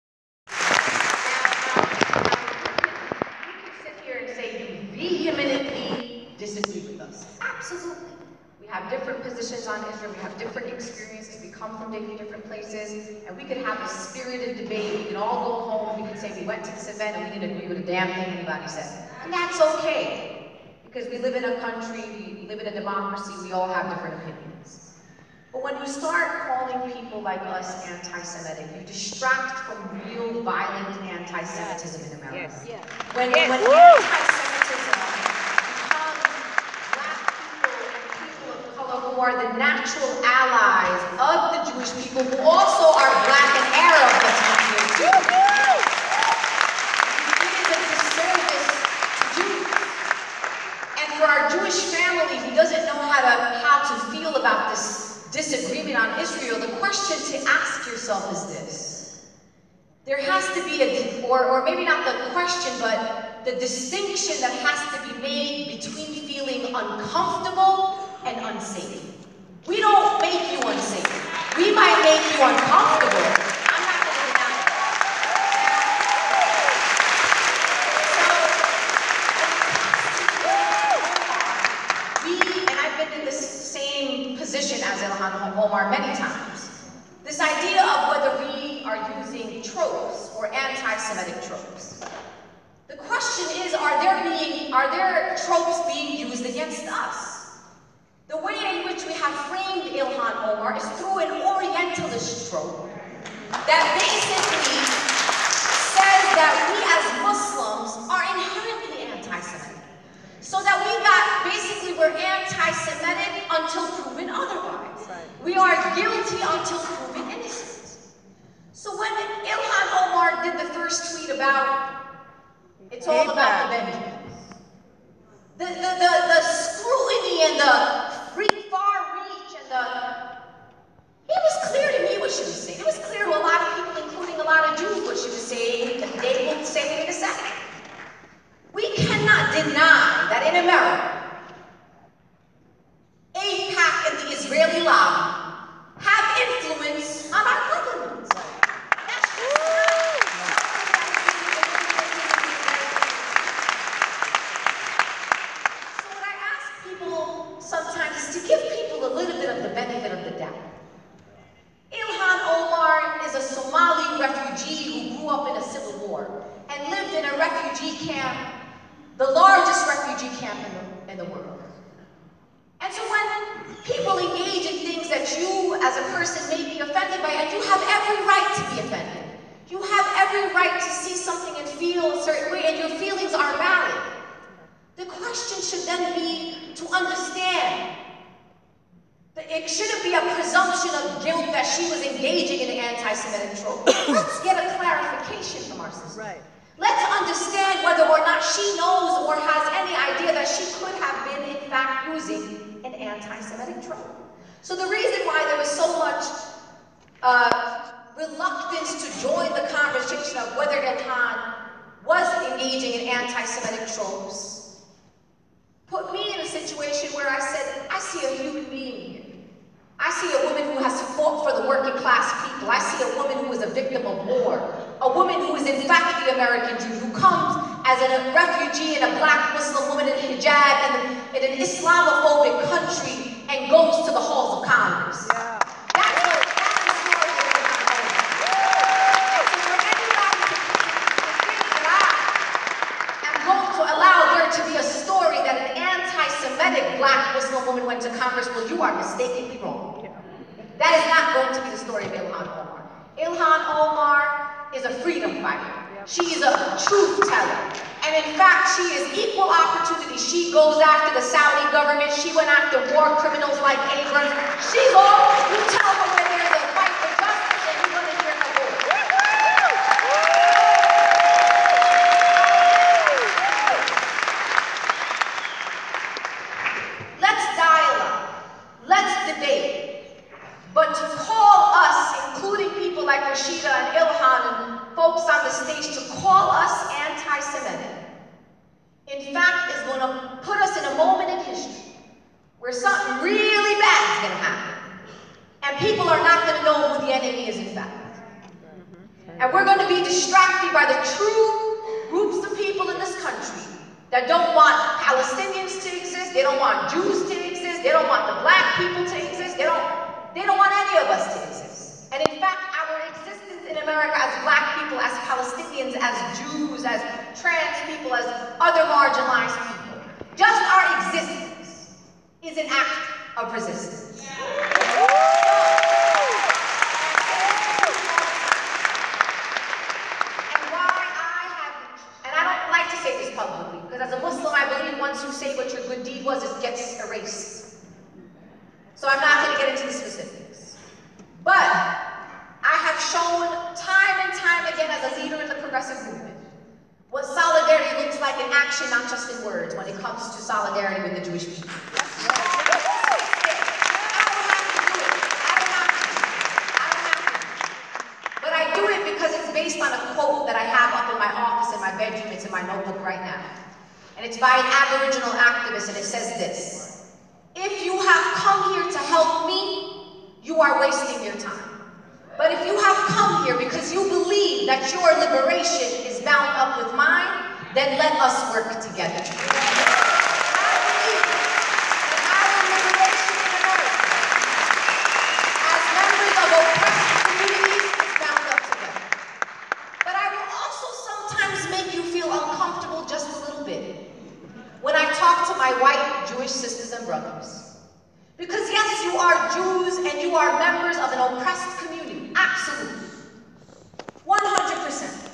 Linda Sarsour @ 'Not Backing Down' 3/3
University of Massachusetts, Amherst, MA
Garret attends the 'Not Backing Down: Israel, Free Speech, and the battle for Palestinian rights' speakers panel at UMass Amherst. Hosted by the Media Education Foundation and Students for Justice in Palestine, panel features Linda Sarsour, Roger Waters, Dave Zirin, and Marc Lamont Hill, moderated by Vijay Prashad. Panel focuses on the human rights situation in Palestine and the slanderous accusations lodged at those who have chosen to speak out in favor of respecting their rights in the Middle East.
Captured using a Samsung Galaxy A tablet  (instead of a Sony HDR-CX240 like it should have been, jah damn TSA).